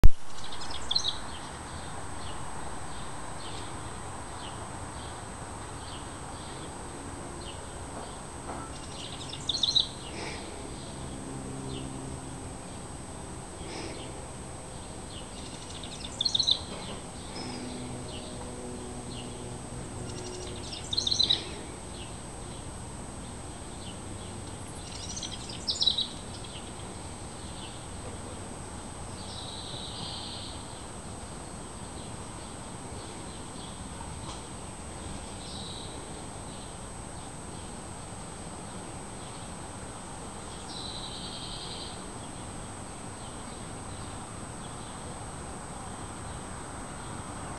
Will Rent for a Song
Wrens building a nest in a wren house